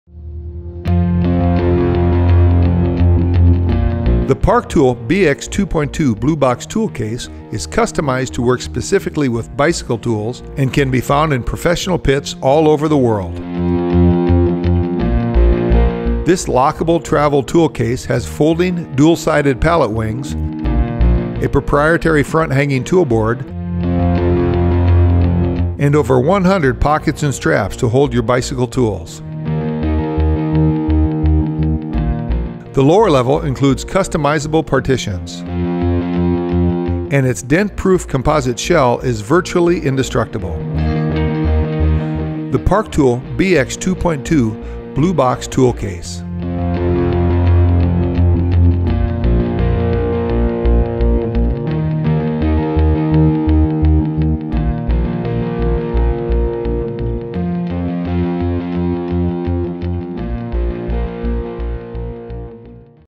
BX-2.2 Blue Box Tool Case
The BX-2.2 features an all-new pallet configuration and storage options to help you keep your tools secure and easily accessible. The expanded custom designed tool pallets, including dual-sided pallet wings, are all housed in a virtually indestructible composite travel case. Designed and built for the professional traveling mechanic who demands the best!

• Custom designed tool-holding pallets with loops and pockets
• Folding, dual-sided pallet wings
• Front tool pallet that stores securely inside during transport
• Dent-proof composite shell built around an aluminum frame
• Key lock
• Nominal dimensions: 19" x 14.85" x 8.5" (48.25 cm x 37.7 cm x 21.6 cm), 13.2 lbs. (6 kg)